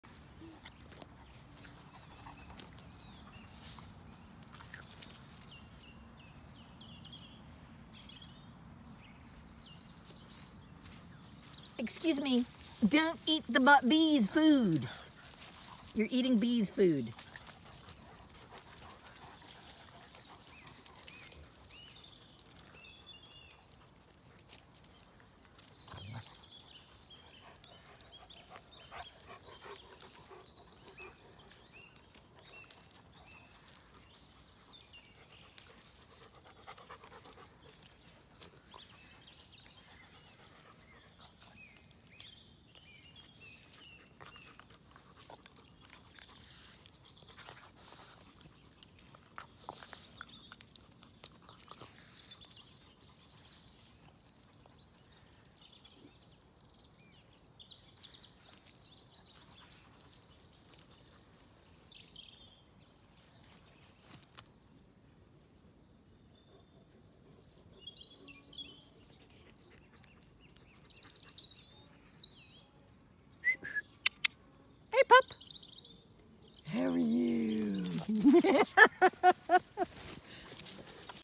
Puppers play amongst the crocuses
Oscar having great fun playing with all the older dogs on this bank of crocuses. Luckily there were no honey bees seeking food while the played.